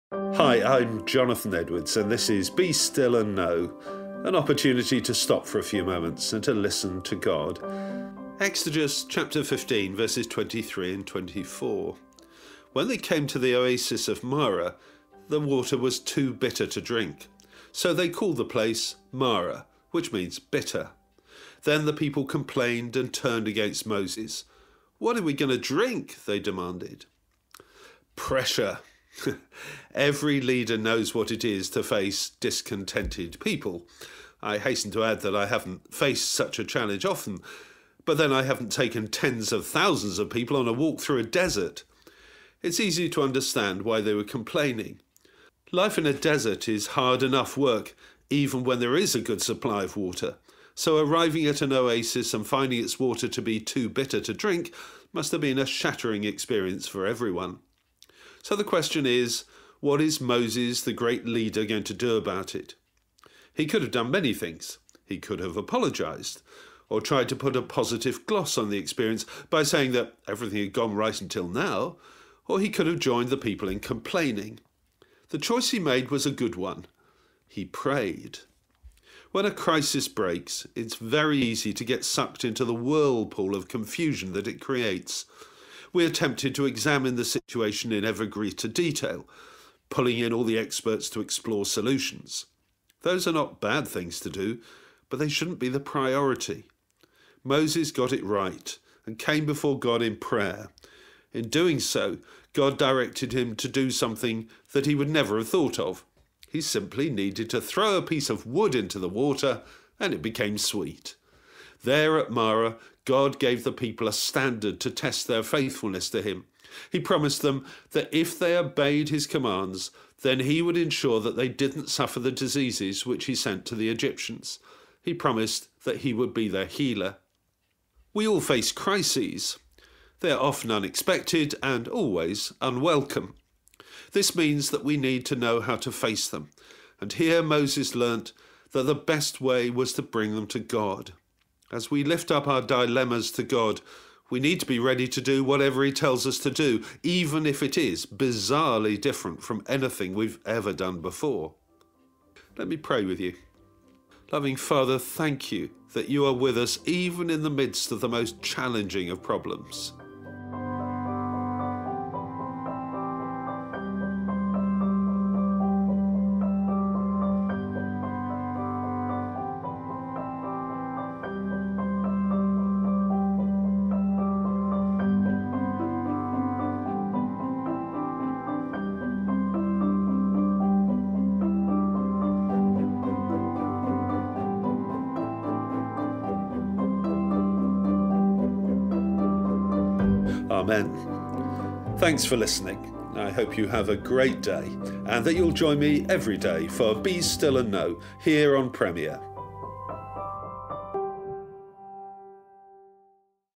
Day 39 - Issue 39
Exodus 15.23-24 NLT

When they came to the oasis of Marah, the water was too bitter to drink. So they called the place Marah (which means “bitter”). Then the people complained and turned against Moses. “What are we going to drink?” they demanded.

Pressure. Every leader knows what it is to face discontented people. I hasten to add that I haven’t faced a huge number, but then I’ve never taken tens of thousands of people on a walk through a desert! It’s easy to understand why they were complaining. Life in a desert is hard enough work even when there is a good supply of water, so arriving at an oasis and finding its water to be too bitter
to drink must have been a shattering experience for everyone. So the question is, what is Moses the great leader going to do about it? He could have done many things. He could have apologised or tried to put a positive gloss on the experience by saying that everything had gone all right until now, or he could have joined the people in complaining. The choice he made was a good one. He prayed.

When a crisis breaks, it is very easy to get sucked into the whirlpool of confusion that it creates. We are tempted to examine the situation in ever greater detail, pulling in all the experts to explore solutions. Those are not bad things to do, but they shouldn’t be the priority. Moses got it right and came before God in prayer. In doing so, God directed him to do something that he would never have thought of. He simply needed to throw a piece of wood into the water and it became sweet. There at Marah God gave the people a standard to test their faithfulness to him. He promised them that if they obeyed his commands then he would ensure that they didn’t suffer the diseases which he sent to the Egyptians. He promised that he would be their healer.

We all face crises. They are often unexpected and always unwelcome. This means that we need to know how to face them, and here Moses learnt that the best way was to bring them to God. As we lift up our dilemmas to God, we need to be ready to do whatever he tells us to do, even if it is bizarrely different from anything that we have done before.

QUESTION: How good are you at handling crises, and how do you think you could improve?

PRAYER: Loving Father, thank you that you are with us even in the midst of the most challenging of problems. Amen